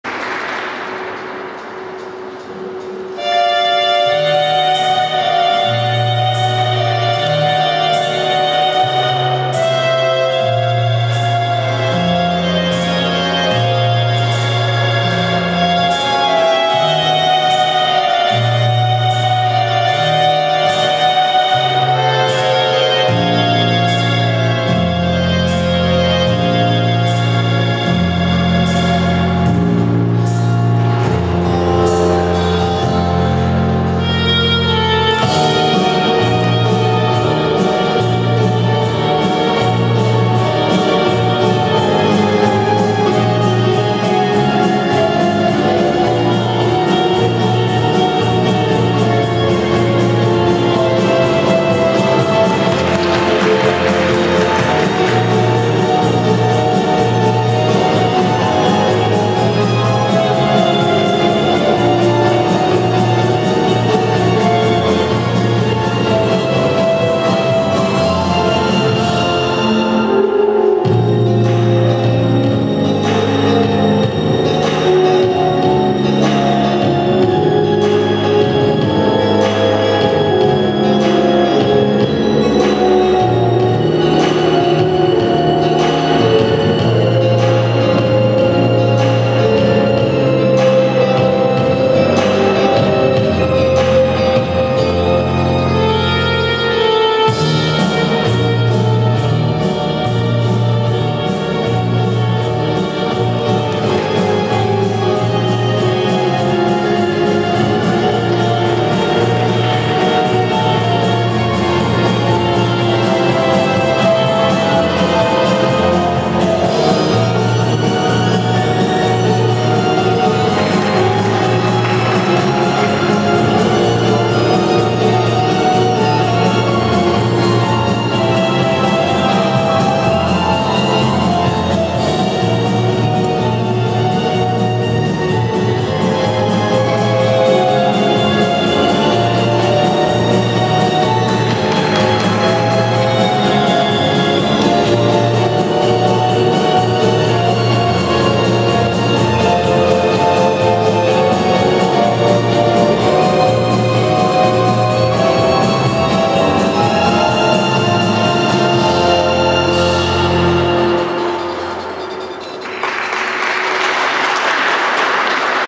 Aleksandra SHLAHOVA, LAT, Cubs A Girls - Free Skating
Volvo Open Cup 20th, Riga, Latvia (Jan2013)